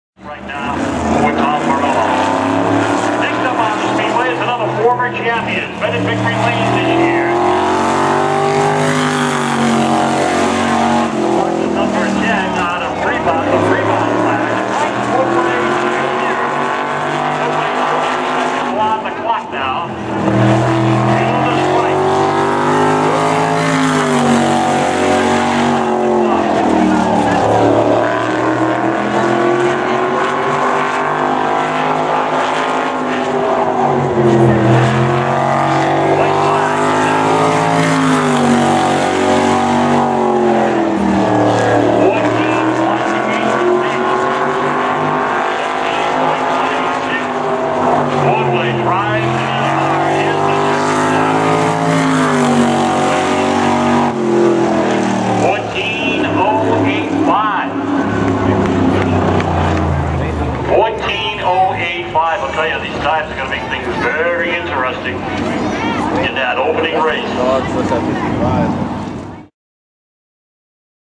Mike Ordway Jr. Qualifying @ Lee USA 7/20/2012
Mike Ordway Jr. time trial qualifying for the 350 Supermodified Moneybags 100 at Lee USA Speedway.

7/20/2012